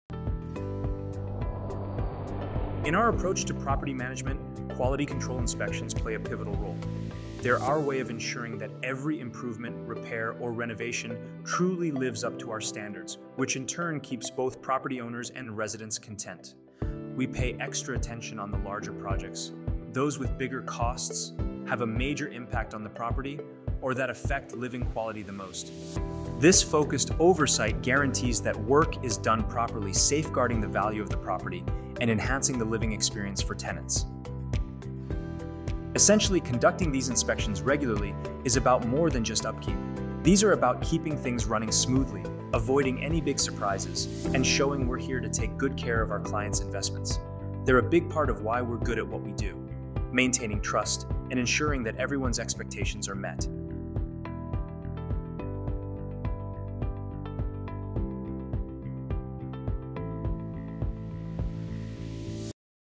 Quality Control Inspections
Building trust, one inspection at a time! Our latest reel takes you inside the rigorous process that ensures every home we touch stands the test of time.

Check this out to see how we're setting new standards in property management.

Want us on your team? Tap the link below to connect!